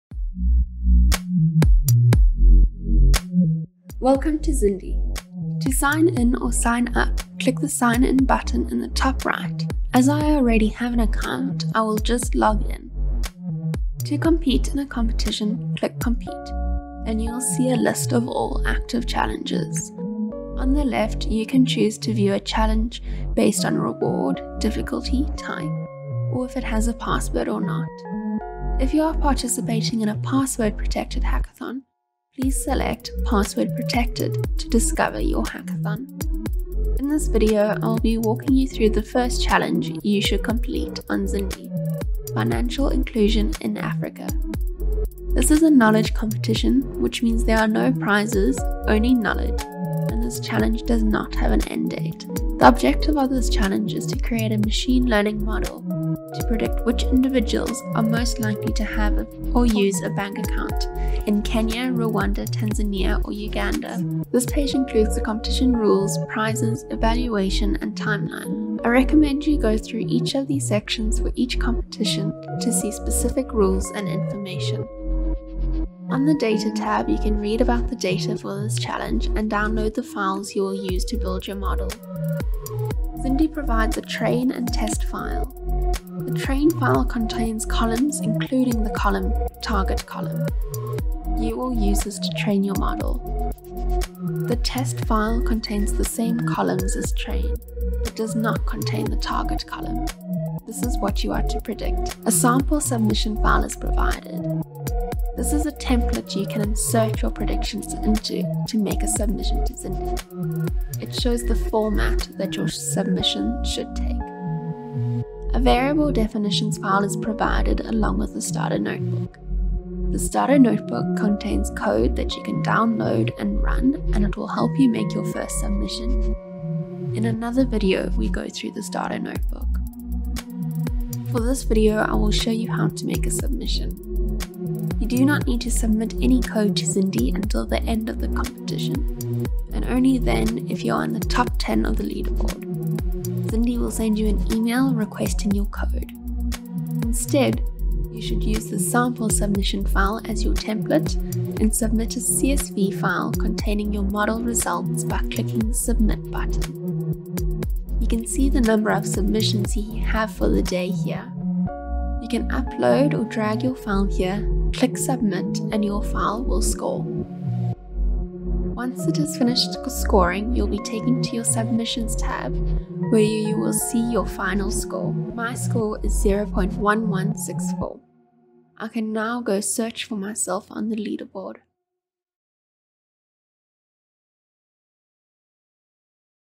How to enrol in your first Zindi competition
Zindi Data Scientist, Amy Bray, shows you how to enrol in a competition and make a submission so that you can KeepLearning and KeepWinning!